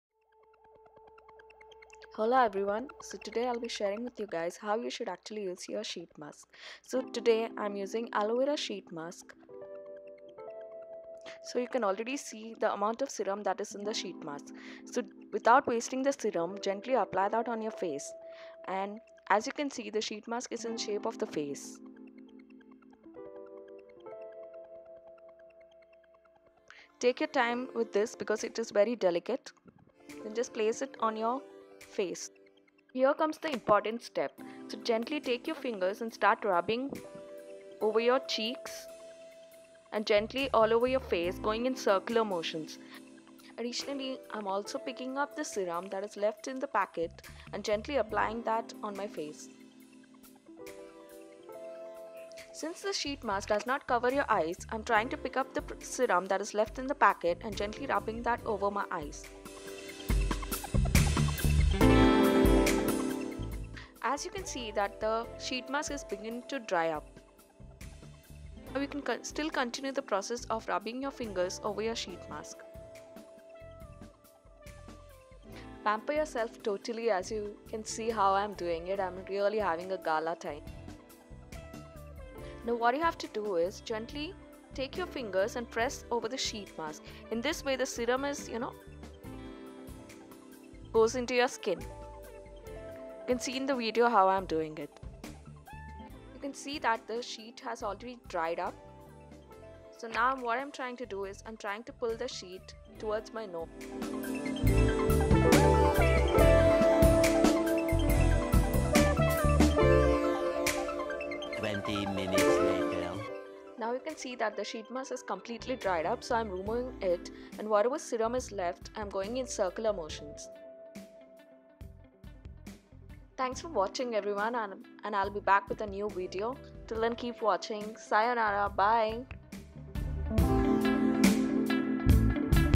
How to use your Sheet Mask | Deepa Sree
Hola,
This is Deepa Sree

Sheet Mask has become almost mandatory in your skin care routine.

But are you using it effectively?

Please watch the video with a little help from my side to know how effectively you should use your sheet mask.